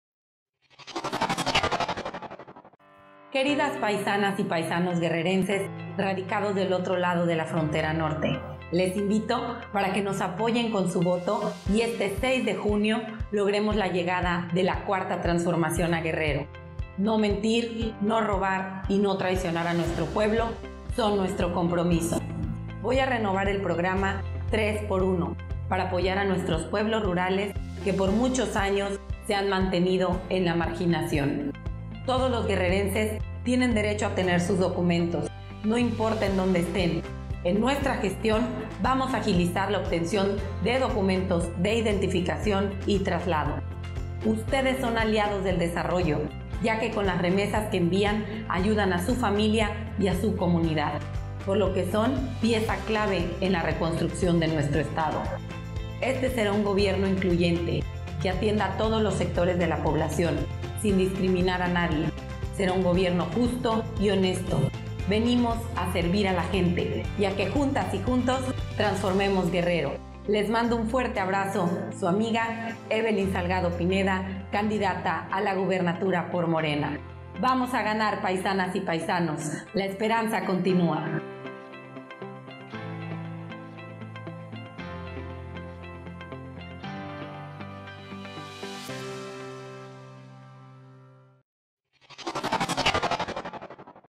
Evelyn Salgado Pineda Candidata de Morena
La candidata de Morena a gobernadora de Guerrero, Evelyn Salgado Pineda, exhortó a guerrerenses que viven en Estados Unidos a votar por ella el 6 de junio para que la 4T llegue al estado